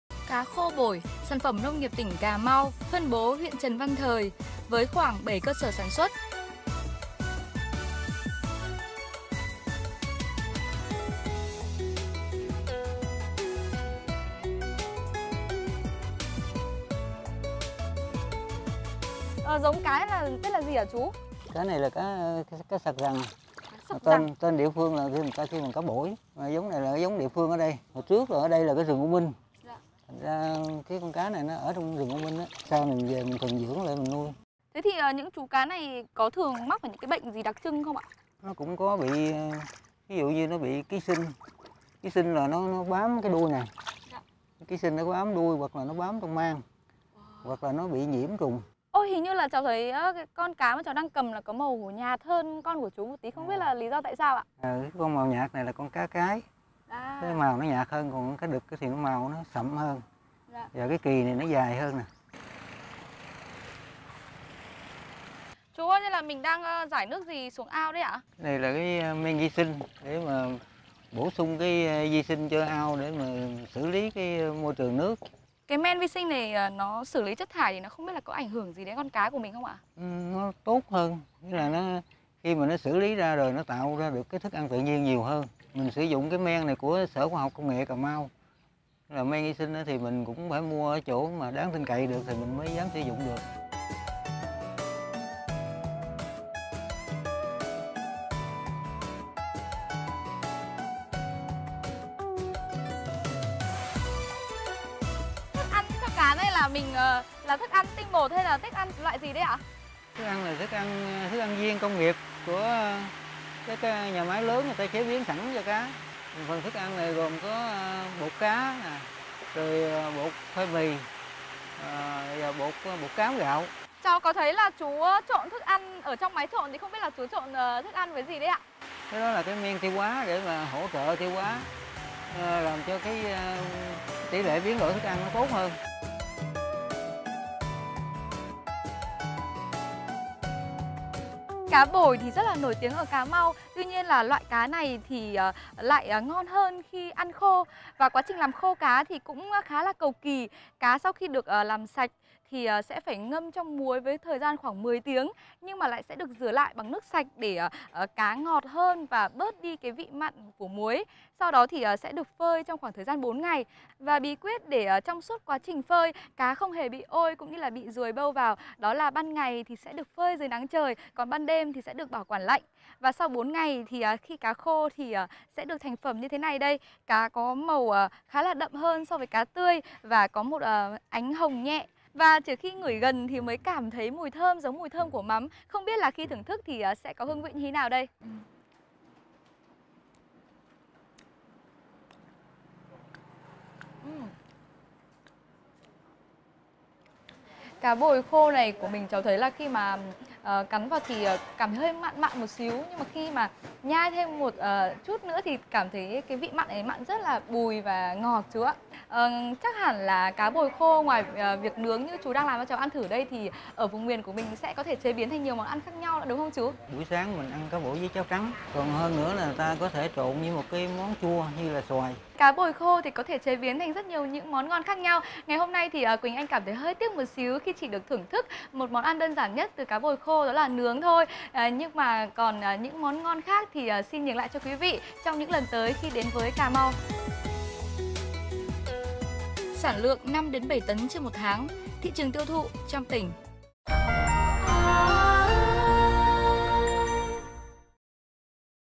NGƯỜI DÂN ĐẤT MÙI DÙNG CÁ BỔI CỦA RỪNG U MINH LÀM CÁ KHÔ
Khô cá bổi nổi tiếng tại Cà Mau có lẽ nhờ vùng U Minh là vùng đất phần lớn còn chìm ngập dưới mặt nước, lại được che phủ bởi rừng tràm trầm thủy, là môi trường lý tưởng cho phù du và rong tảo sinh sôi, nên chất lượng con cá cao nhất. Để tăng sản lượng cá, người dân Cà Mau đã mang giống cá này về nuôi trong ao nhà theo mô hình an toàn. Sau đó, cá được phơi khô theo phương pháp đặc biệt nhằm bảo quản được lâu, nâng cao giá thành sản phẩm.